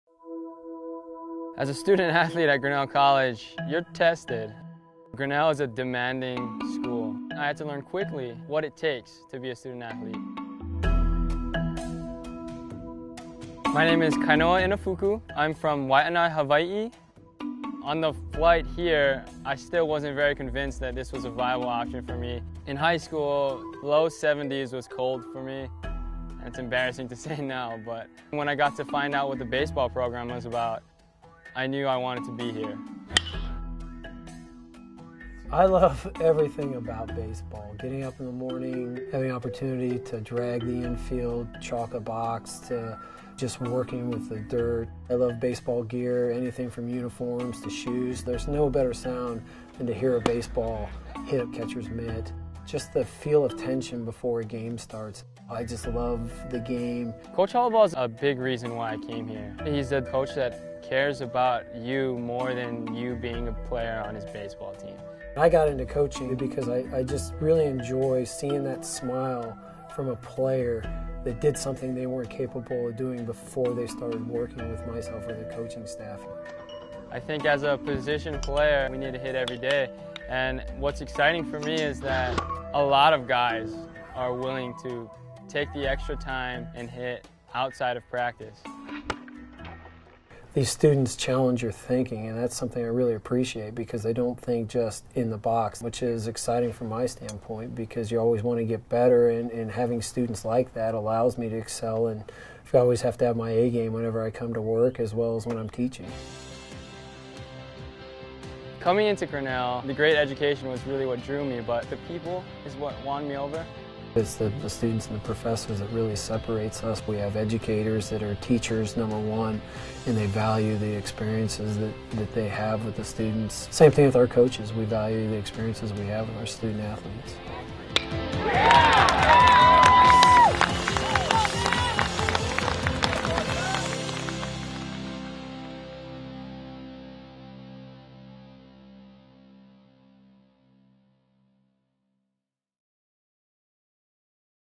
Student and Athlete
Not just a student. Not just an athlete. How do you balance a great education and academic rigor with strong athletics?

For Kainoa Inafuku — as with many student-athletles --  it was with professors who demanded his best and helped him deliver. With coaches who care about their players beyond the field. With teammates who supported and challenged him.